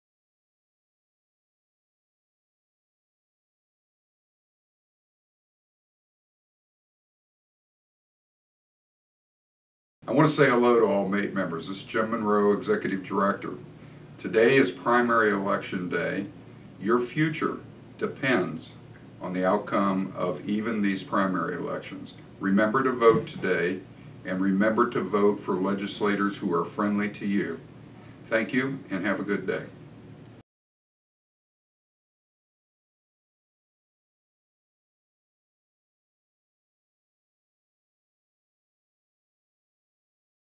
Primary Day voting reminder
Today is the Primary Election! Don't forget to vote for labor-friendly legislators.
The outcome of the election and the future of your jobs depend on it!